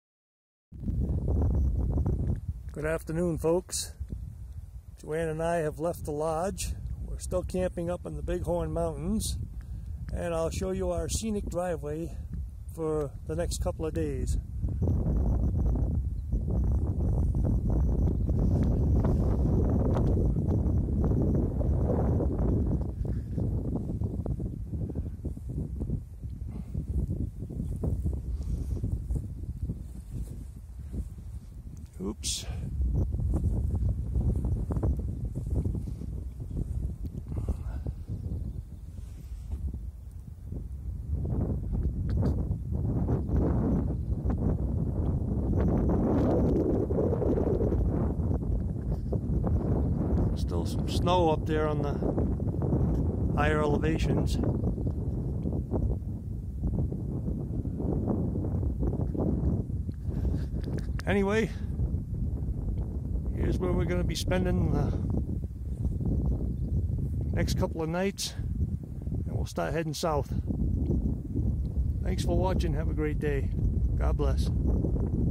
Full Time RVing - Bighorn Mountains , Wyoming
We did some dispersed camping on the Bighorn Mountains last weekend.